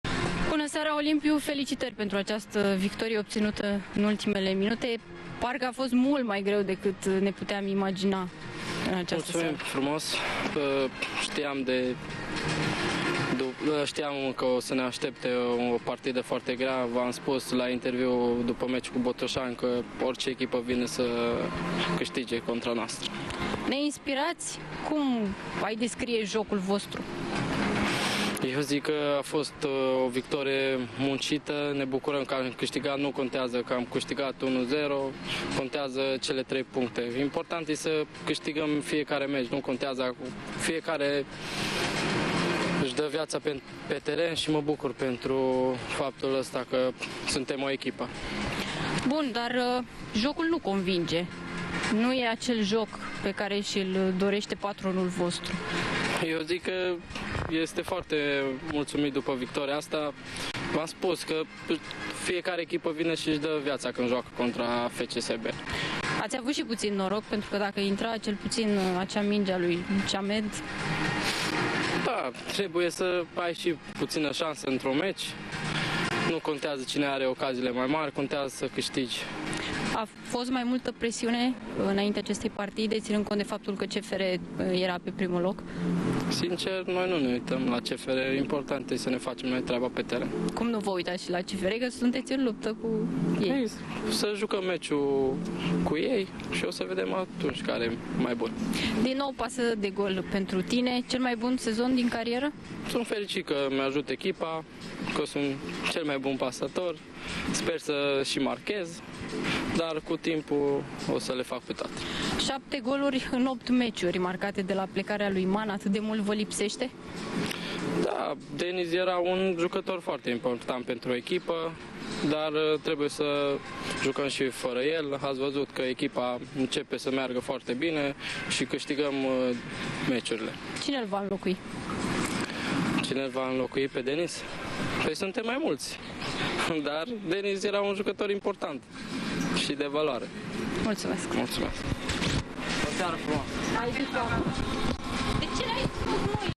INTERVIU. Olimpiu Moruţan o aşteaptă pe CFR: "Să vedem care-i mai bun"
Mijlocaşul FCSB-ului aşteaptă derby-ul cu marea rivală.